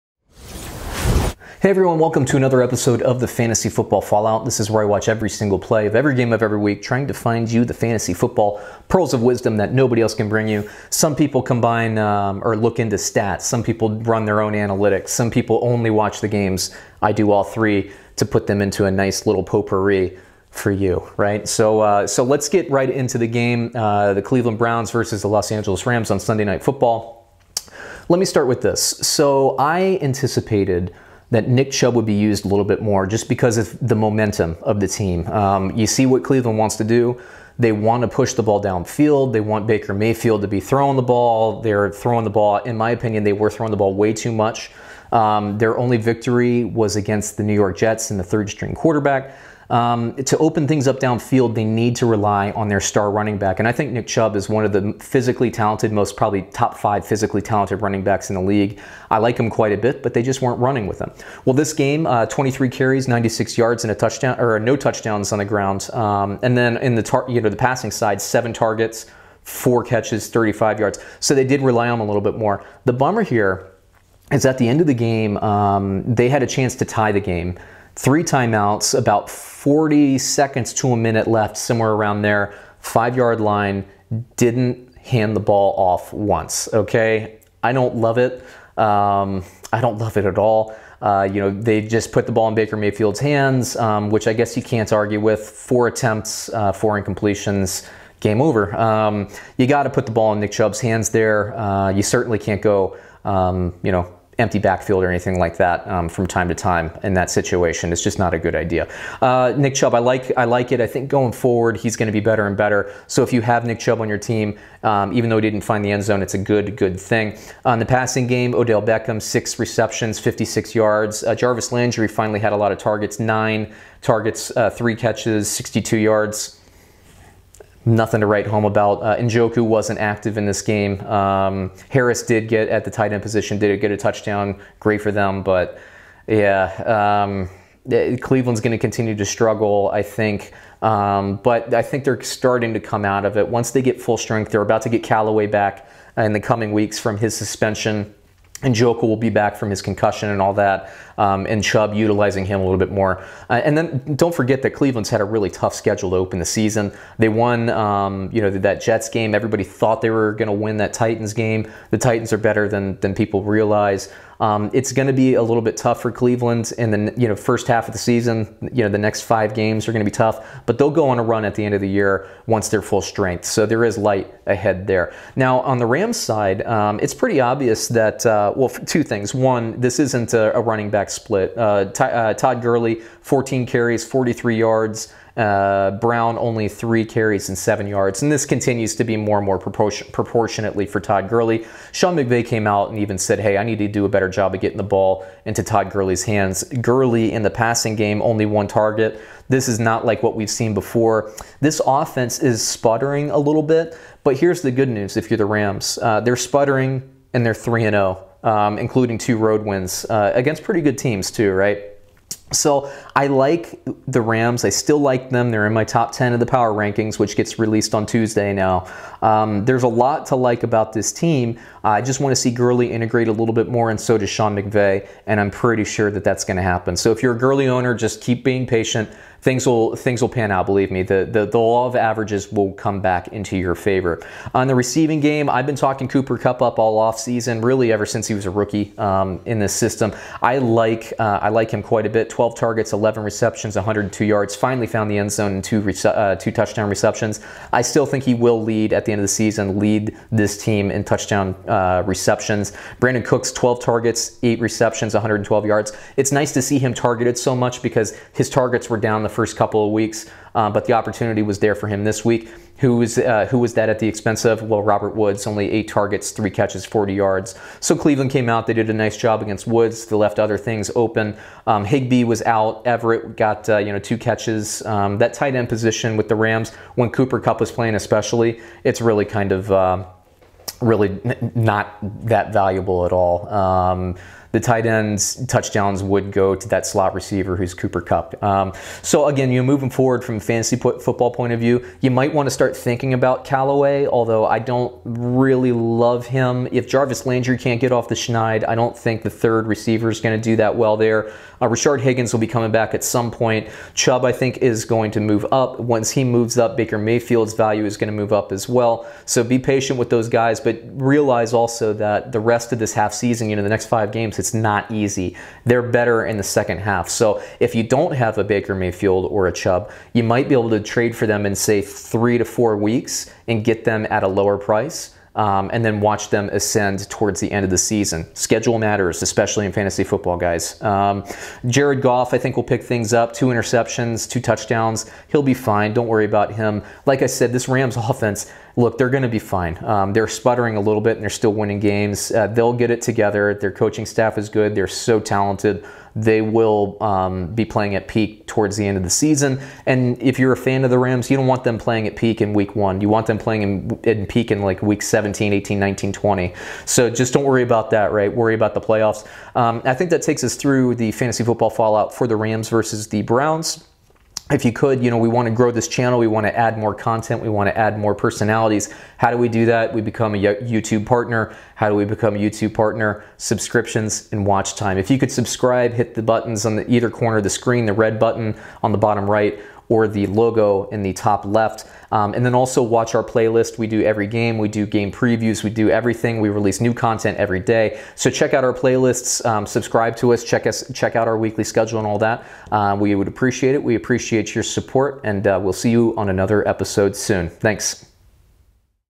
LA Rams vs Cleveland Browns Reaction | Fantasy Football Fallout
The Cleveland Browns defense did well holding the LA Rams offense in check, but it wasn't enough to win the game.

Will Todd Gurley return to fantasy football dominance or will his struggles continue?  Will Sean McVay utilize him more in the backfield?

And despite an increase in carries, Nick Chubb was ignored on the goal line.  Why wouldn't they use him more in scoring situations?